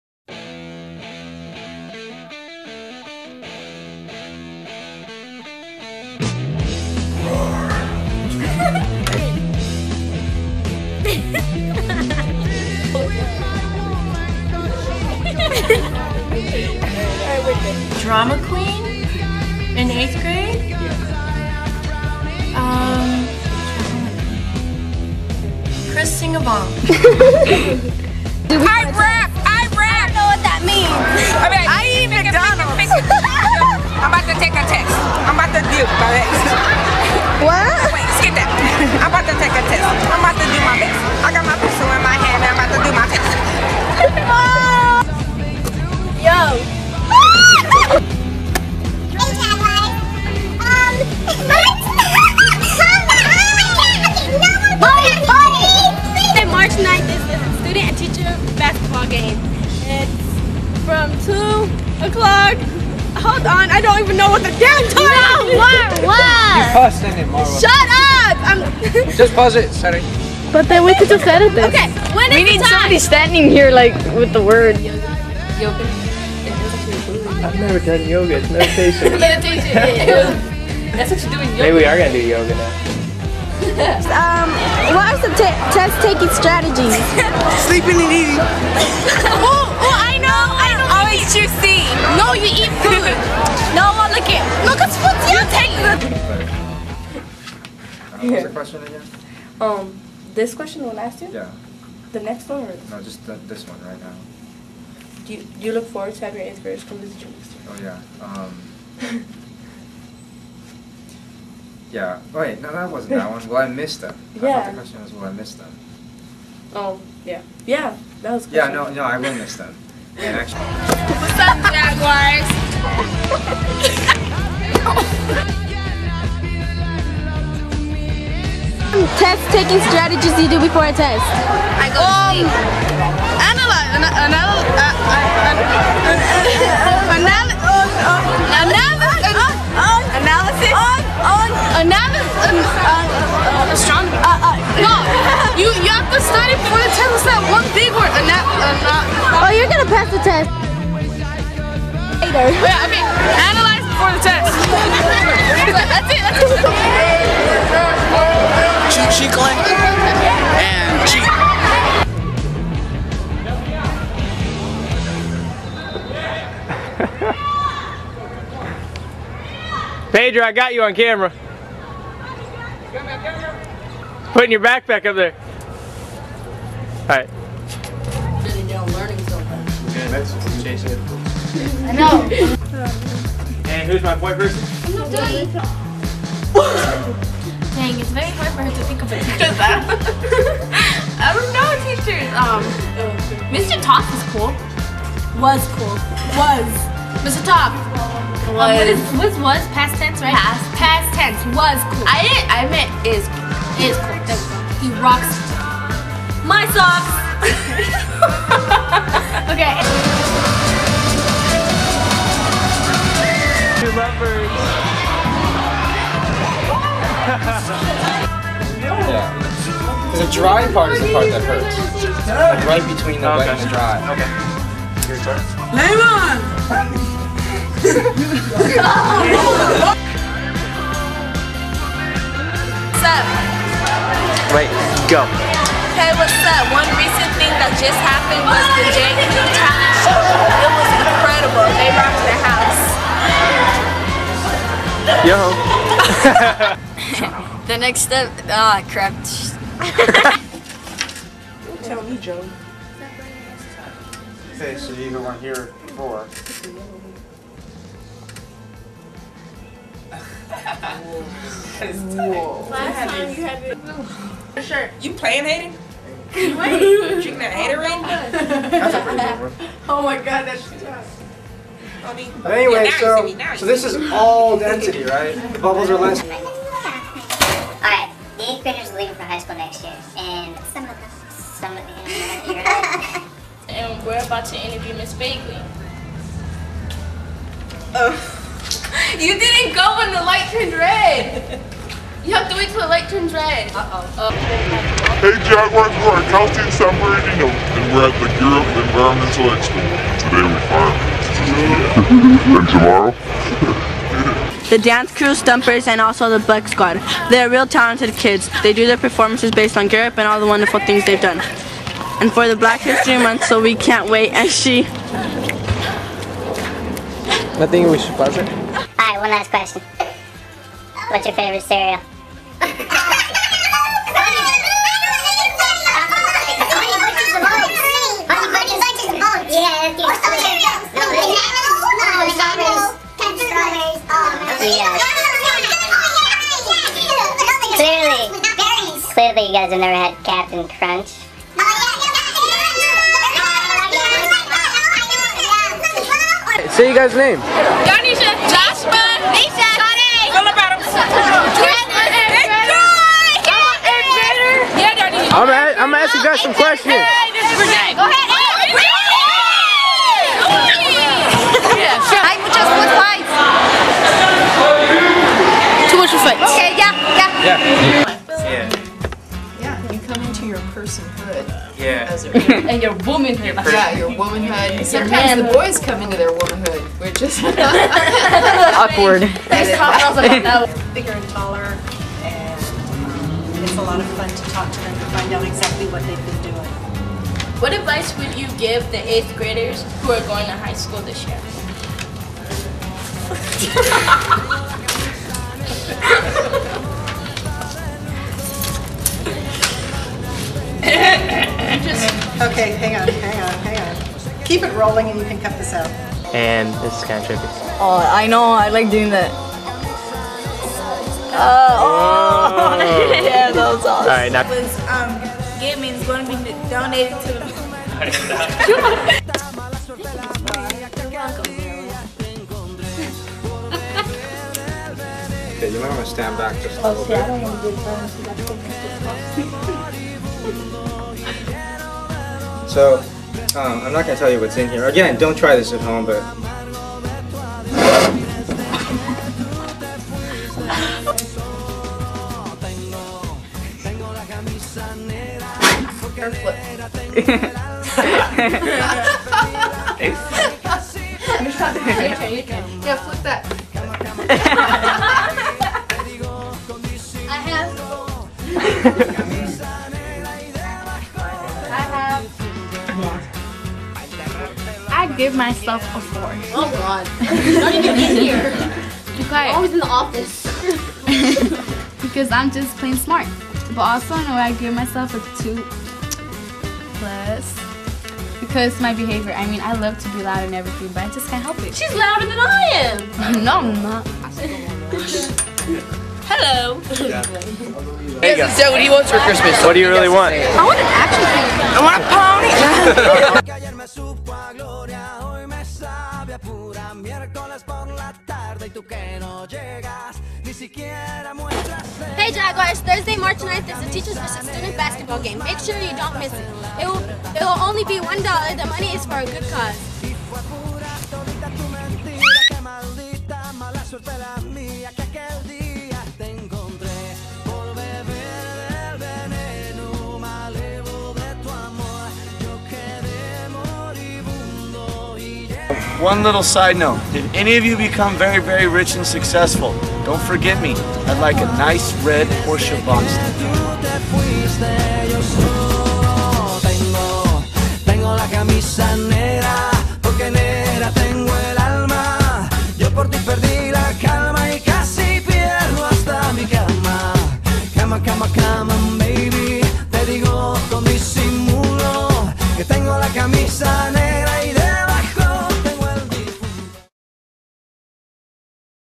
Bloopers (2005-2006)
These are the bloopers that Jaguar Productions created in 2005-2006.